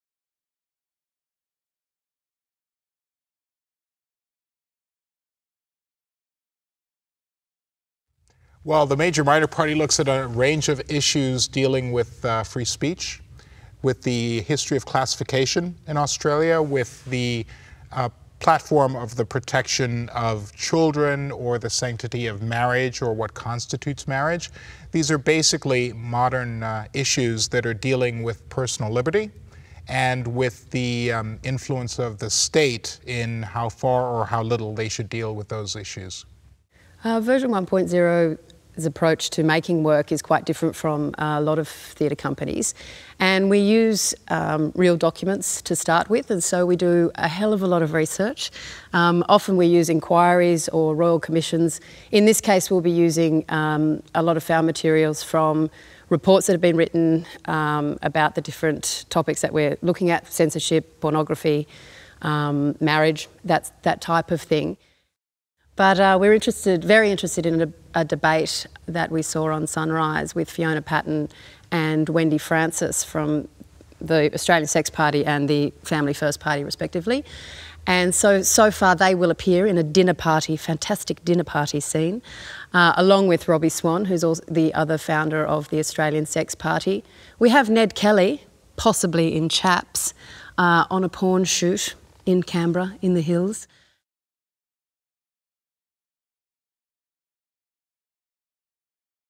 The Major Minor Party - Irving Gregory & Jane Phegan
29 May - 1 June 2013

SEX! now that we have your attention...

Introducing version 1.0, Australia's masters of political-based verbatim theatre. Having tackled the Cole Inquiry, Wollongong Council and SIEV-X disaster, it's now time for The Sex Party!

A world premiere from this Helpmann Award-winning team, The Major Minor Party explores the rise of the ACT-based Australian Sex Party.
Founded in 2009 by Fiona Patten and Robbie Swan of the Eros Association, an adult industry lobby group, The Australian Sex Party has contested three elections, winning enough votes in 2010 to feel confident to declare themselves "the major minor party" in Australian politics.

In a political climate increasingly dominated by pseudo-religious and nationalist parties such as Family First or One Nation, this performance charts the emergence of The Australian Sex Party, as they expose hypocrisy, fight against censorship, host brothel open days, and take visiting dignitaries on guided tours of the porn industry.

Densely interweaving irreverent hilarity and sobering seriousness, The Major Minor Party is a wild and eye-opening ride through the Australian political landscape.